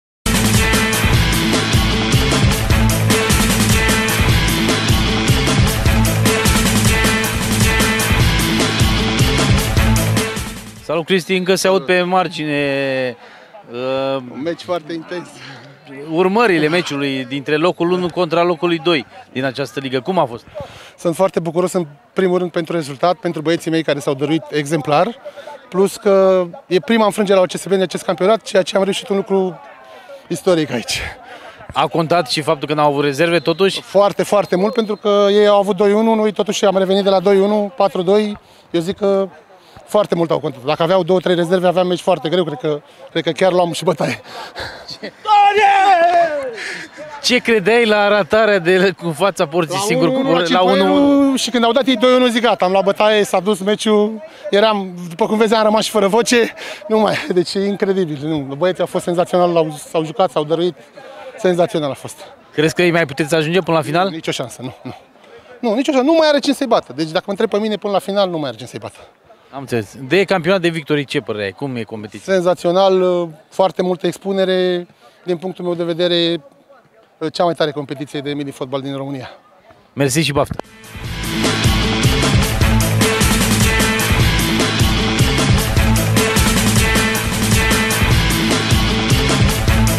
Interviu Doborgeanu Cristinel - Simple | Meciul Săptămânii la Victory Cup
⚪️ 𝑫𝒐𝒃𝒓𝒐𝒈𝒆𝒂𝒏𝒖 𝑪𝒓𝒊𝒔𝒕𝒊𝒏𝒆𝒍 este extrem de fericit după victoria echipei sale, după un final de meci fantastic! 🔥

⚫️ 𝑺𝒊𝒎𝒑𝒍𝒆 este prima echipă care învinge liderul ASCB în acest sezon și începe să creadă tot mai mult în puterile lor pentru promovarea în liga secundă!💪🏼

👏Felicitări, băieți pentru victoria din 𝐌𝐄𝐂𝐈𝐔𝐋 𝐒𝐀̆𝐏𝐓𝐀̆𝐌𝐀̂𝐍𝐈𝐈, iar interviul integral este mai jos 👇🏼